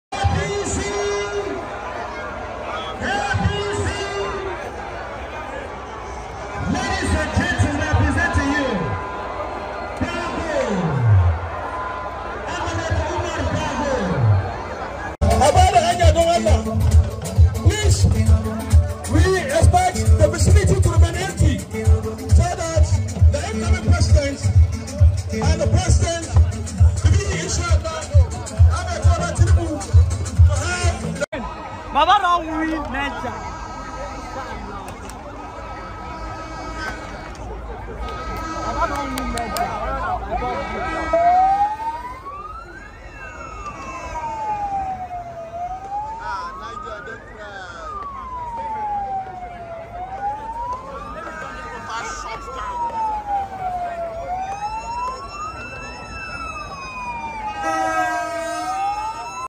2023 : Magnificent Edifice Donated To The Tinubu Campaign By Senator Sani Mohammed In Niger State
2023 : Magnificent Edifice Donated To The Tinubu Campaign By Senator Sani Mohammed In Niger State